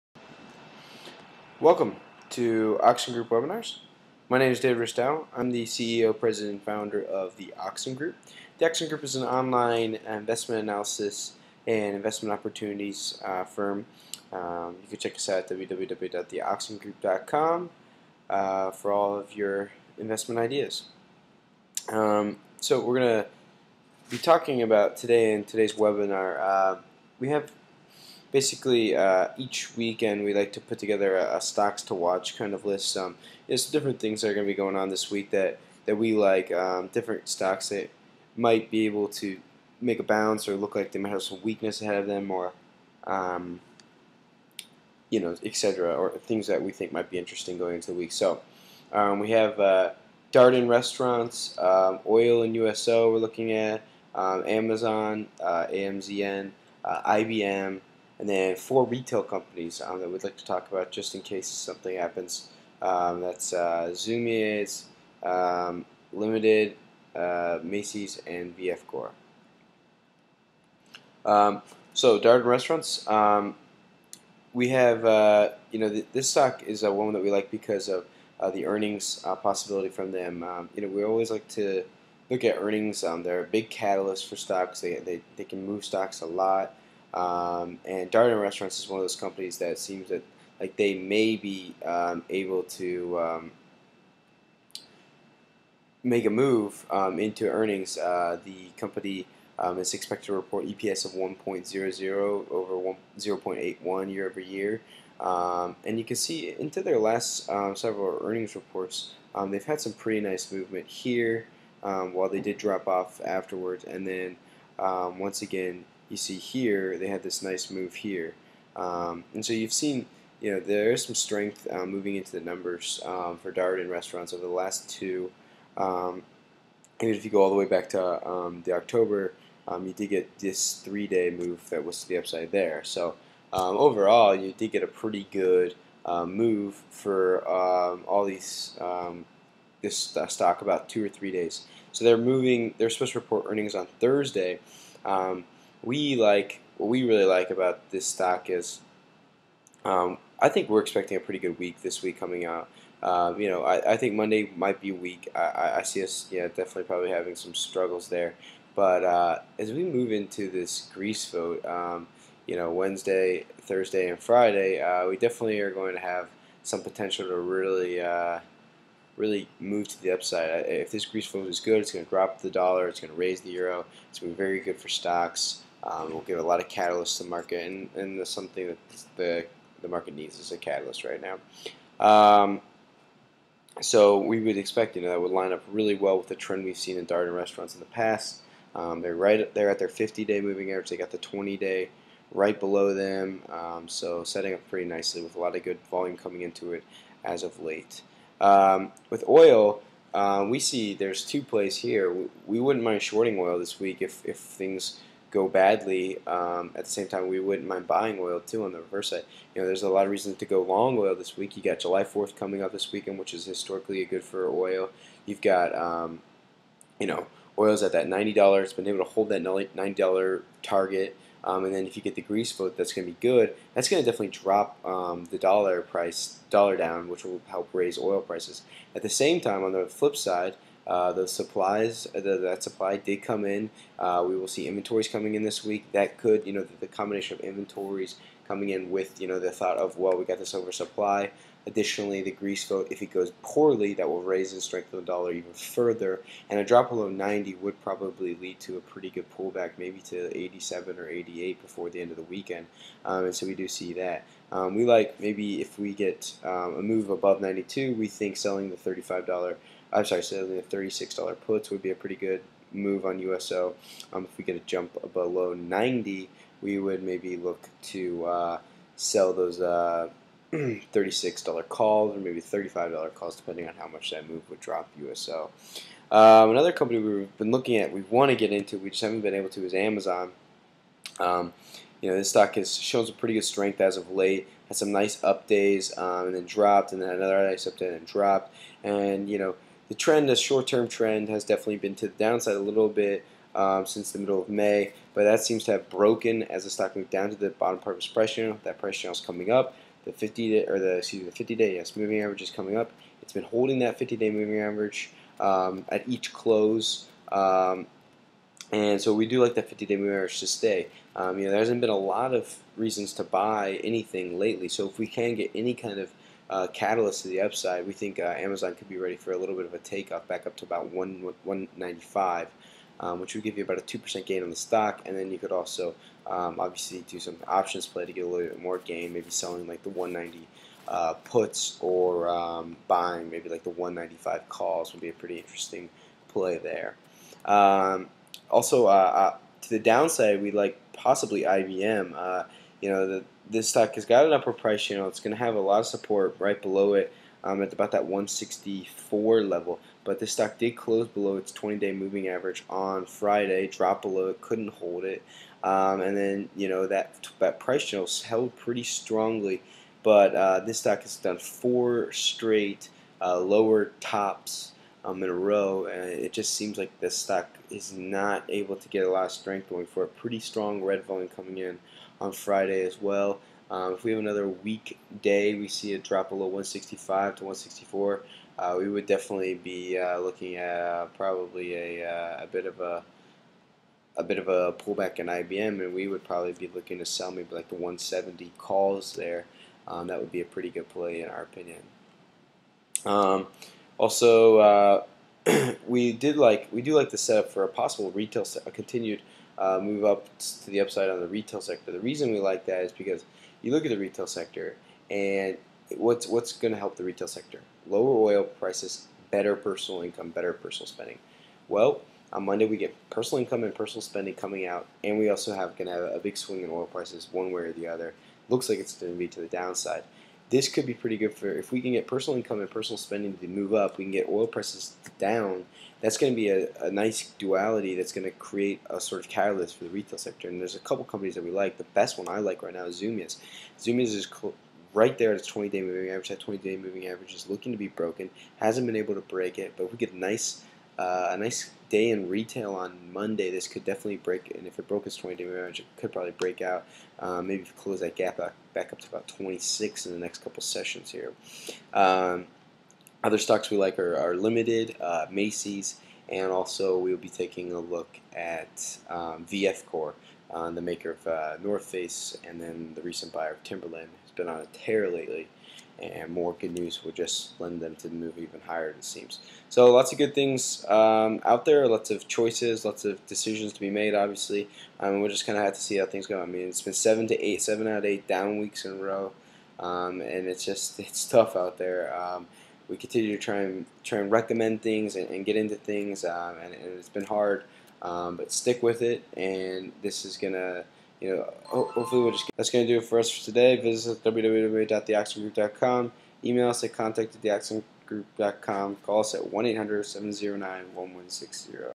Oxen Group Webinar - Stocks to Watch This Week
The Oxen Group looks at stocks to watch this week including DRI, USO, IBM, AMZN, and retail stocks.